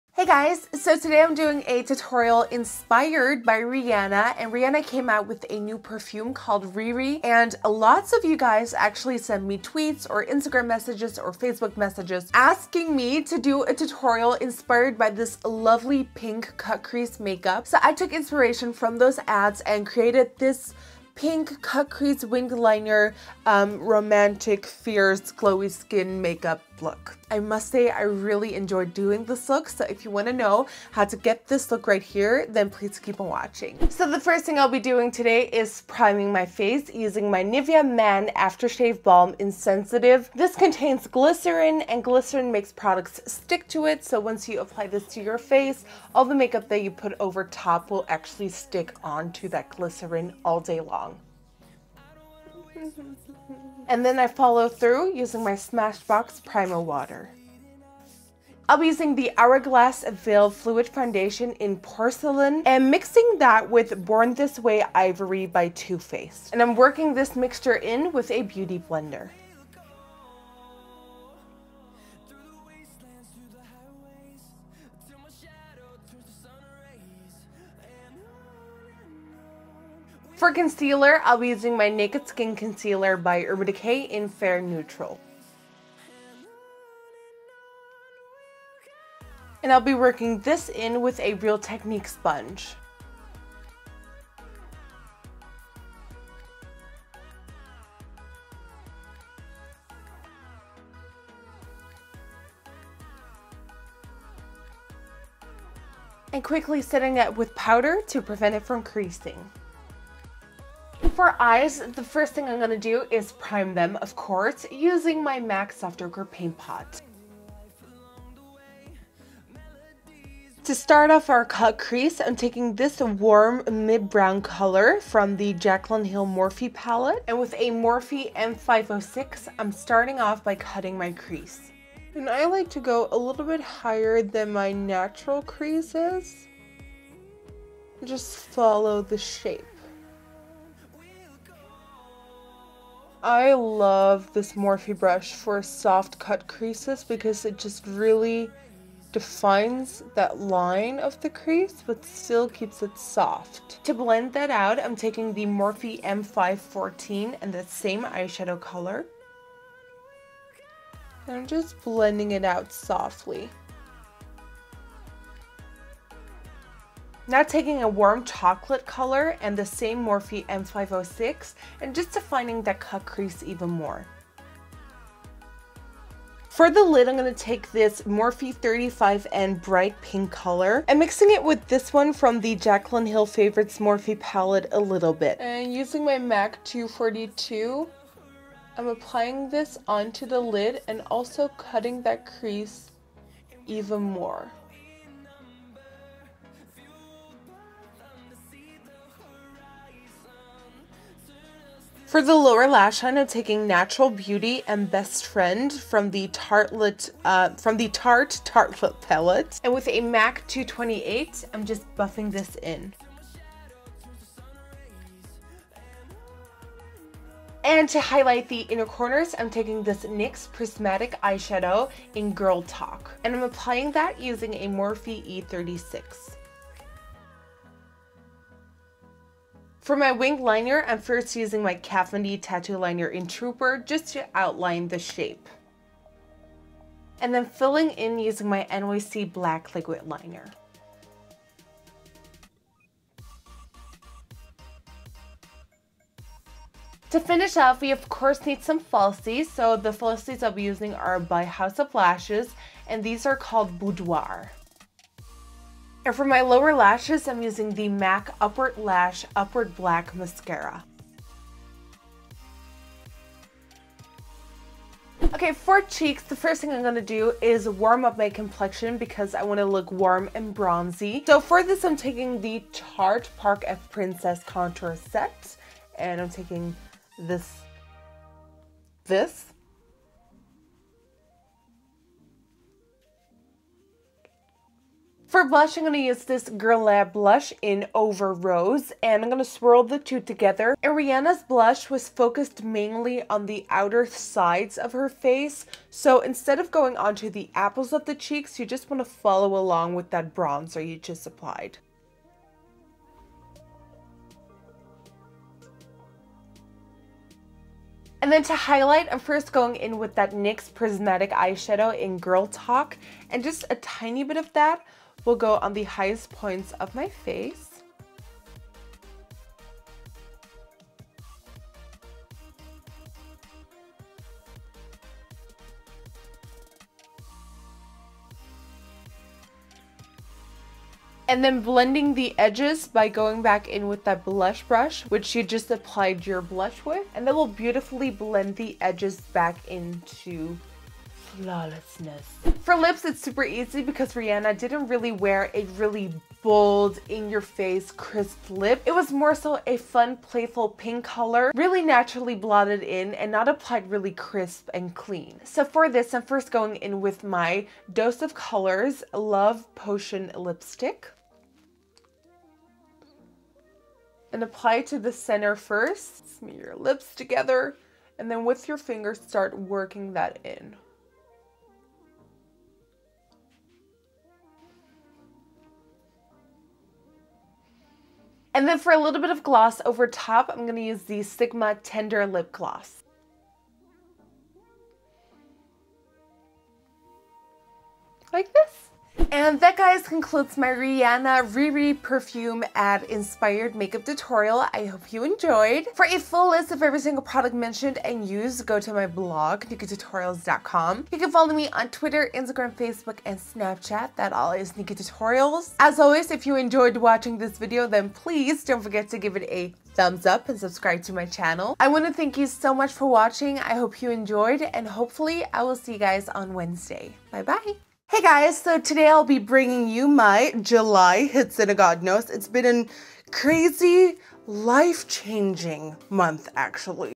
RiRi by Rihanna Inspired Pink Cut-Crease Makeup
Today I’ll show you how to recreate this gorgeous pink cut-crease makeup look inspired by Rihanna's new fragrance RiRi!

SNAPCHAT » Nikkietutorials